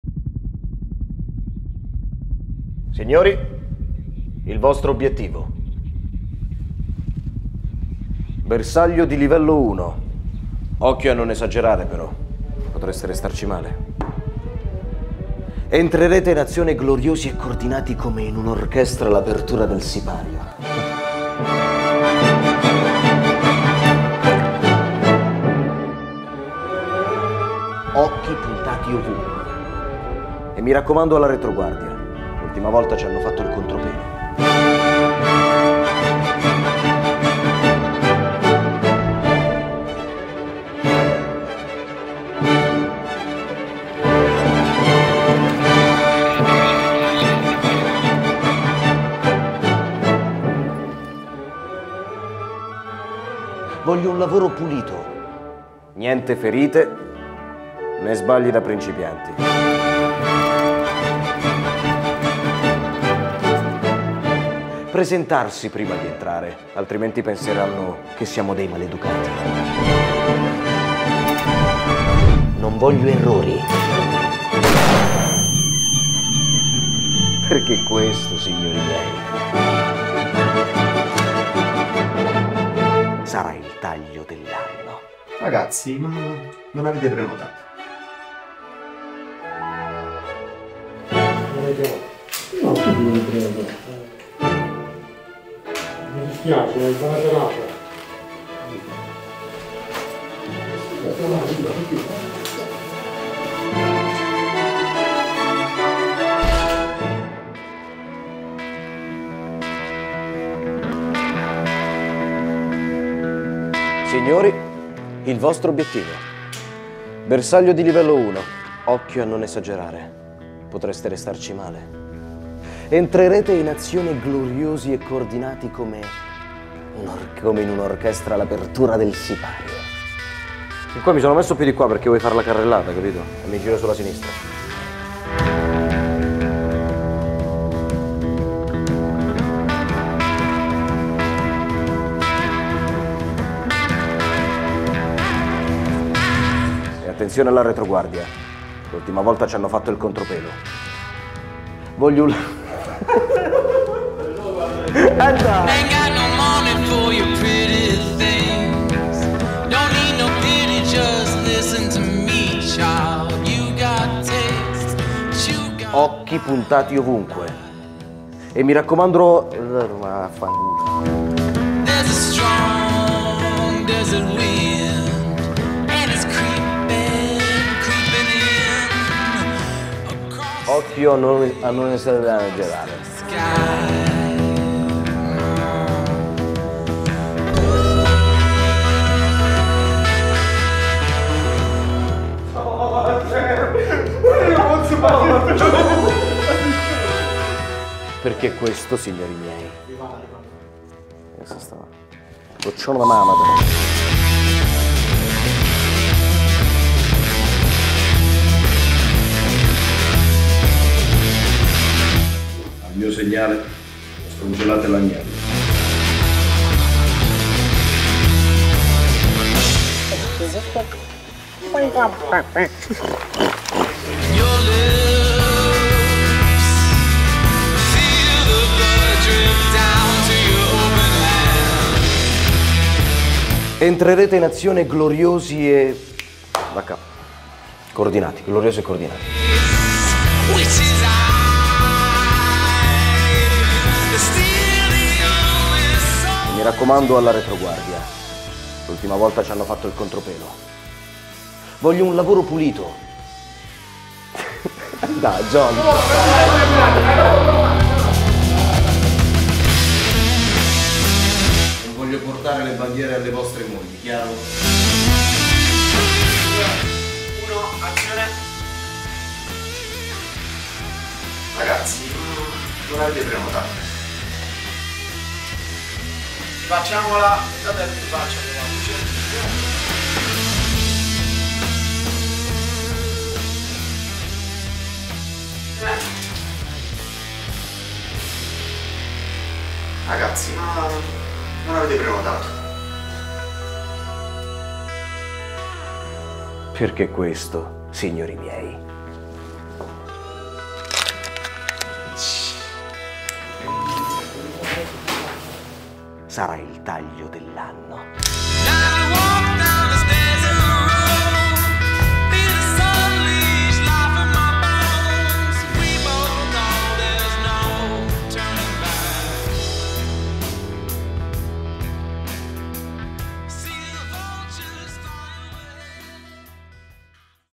BARBER BREACH | Behind the Scenes
Dietro le quinte di BARBER BREACH.

short film by Giovanni di Gennaro
direct by Rocco Allegretti
backstage - Stefano Giansanti
Barber - Pasquale Coppa
Soldier Boss - Manuel Santagata
Soldiers -  Giovanni Di Gennaro ,Luca Summa , Michele Di Tolve

Special thanks @rangerairsoftarmy8601

Camera : SONY ALFA 7 IV

Editing and Color in Davinci Resolve 17